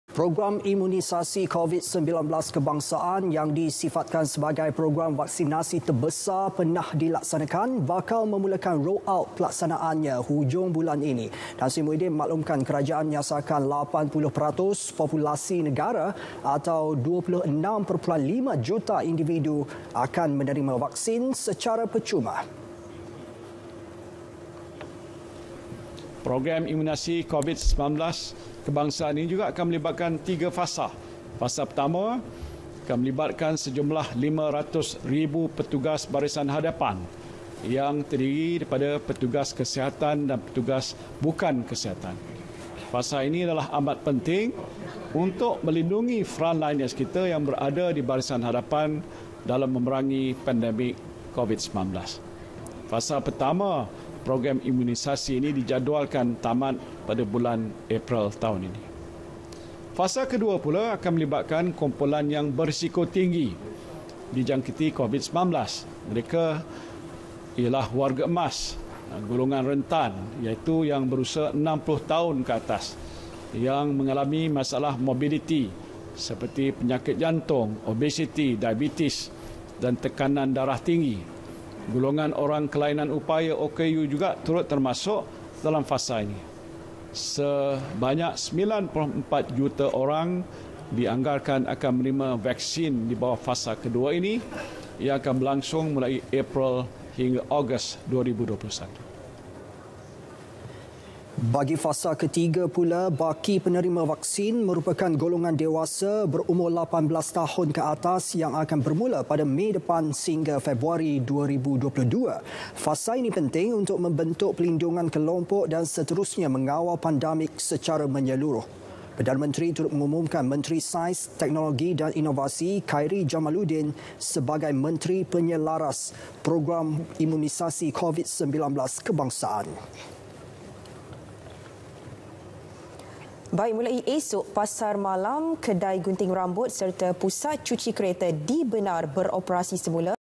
Perlaksanaan Imunisasi COVID-19 Kebangsaan Mulai Hujung Bulan Ini
Program Imunisasi COVID-19 Kebangsaan yang disifatkan sebagai program vaksinasi terbesar pernah dilaksanakan, bakal memulakan 'roll out' perlaksanaannya hujung bulan ini.

Tan Sri Muhyiddin memaklumkan, kerajaaan menyasarkan 80% populasi negara atau 26.5 juta individu akan menerima vaksin secara percuma.
Dan bagi fasa ketiga pula, baki penerima vaksin merupakan golongan dewasa berumur 18 tahun ke atas yang akan bermula pada Mei depan hingga Februari 2022.

Fasa ini penting untuk membentuk perlindungan kelompok dan seterusnya mengawal pandemik secara menyeluruh.
Perdana Menteri turut mengumumkan Menteri Sains, Teknologi dan Inovasi, Khairy Jamaluddin sebagai Menteri Penyelaras Program Imunisasi COVID-19 Kebangsaan.

Sumber: @BeritaRTMBES